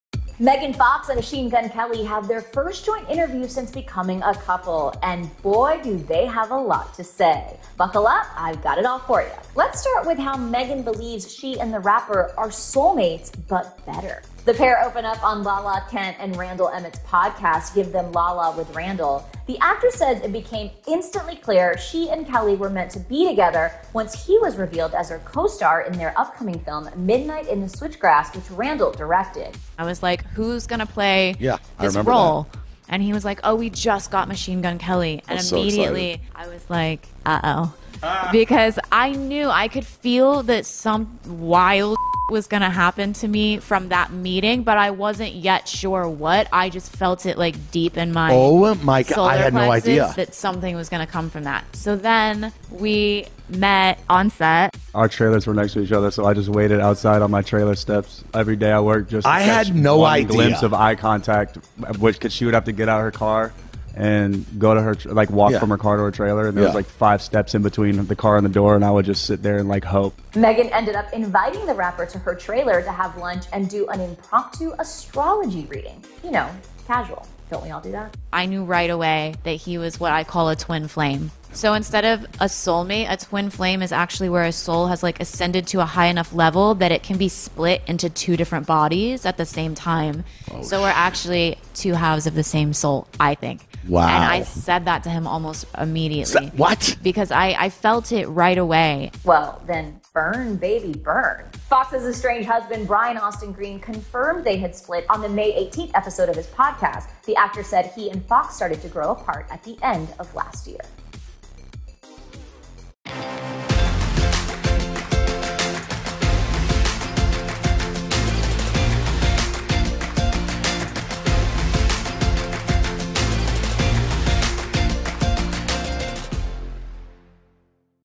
Megan Fox & MGK Are "Two Halves of the Same Soul" | E! News
The "Midnight in the Switchgrass" actress & costar Machine Gun Kelly explain their instant chemistry and call each other twin flames. Watch!